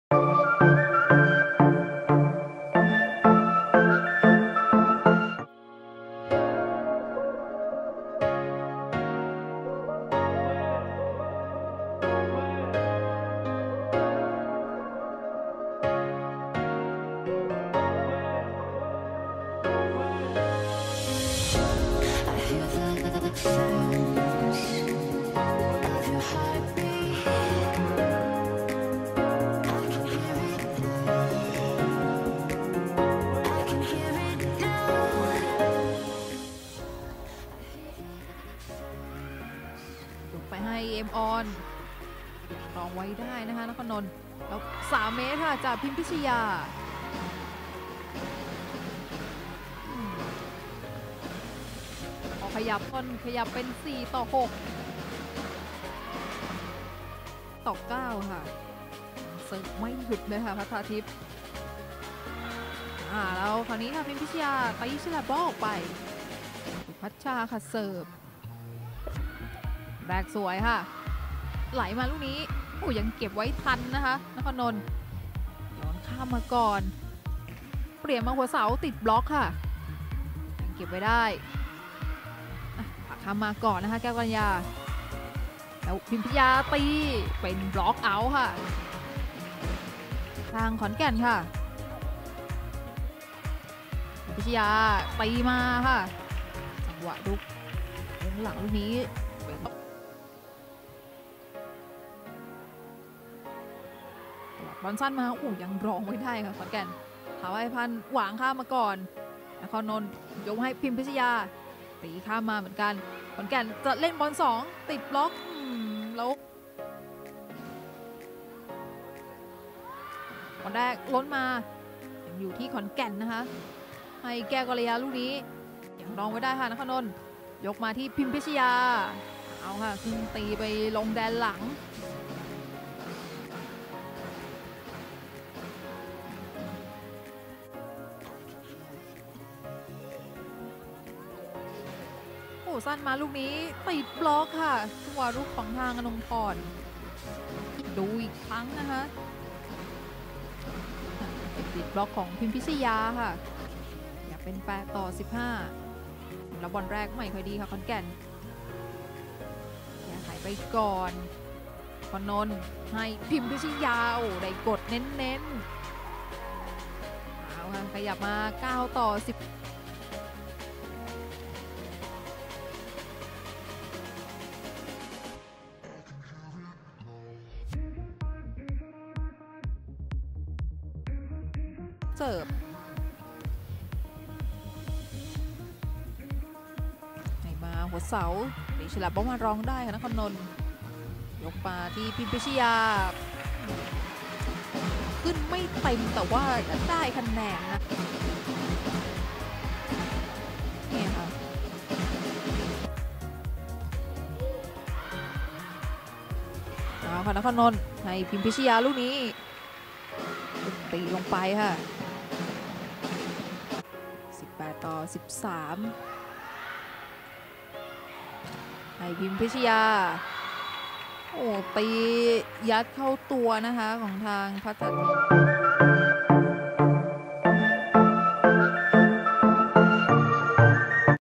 นัดแรกของบีม ฤดูกาล2021 VS ขอนแก่นสตาร์ | พิมพิชญา ก๊กรัมย์ (Pimpichaya Kokram) | Levoellyth
(Hilights) บีม พิมพิชญา ก๊กรัมย์  หมายเลข 12 ตัวตบบีหลัง 3BB นครนนท์
Beem Pimpichaya Kokram No.12 Opposite hitter 3BB Nakornnont.
ไดกิ้น วอลเลย์บอลไทยแลนด์ลีก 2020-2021 ขอนแก่นสตาร์ วีซี พบ 3BB นครนนท์
Daikin Volleyball Thailand League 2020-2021 Khonkaen Star VC vs 3BB Nakornnont.